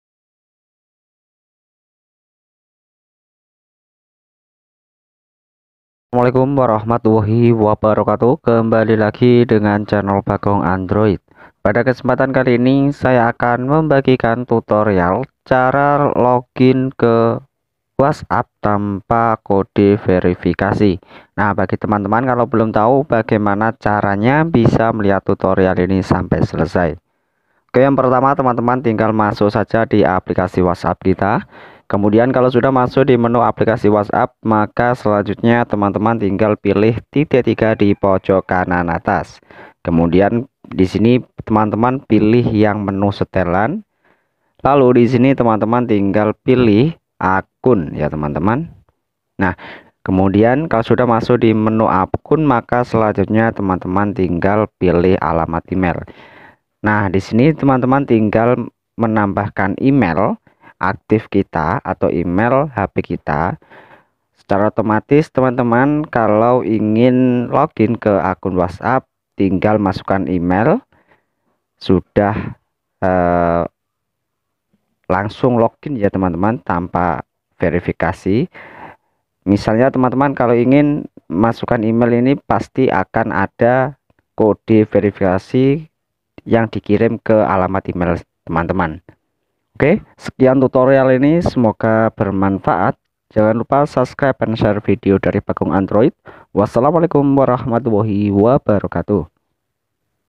Cara Login WhatsApp Tanpa Kode Verifikasi 2024
Halo teman teman bertemu lagi dengan channel Bagong Android pada kesempatan kali ini saya akan membagikan tutorial Cara Login WhatsApp Tanpa Kode Verifikasi 2024

Nah Cara Login WhatsApp Tanpa Kode Verifikasi Terbaru tonton terus video kali ini sampai selesai ya teman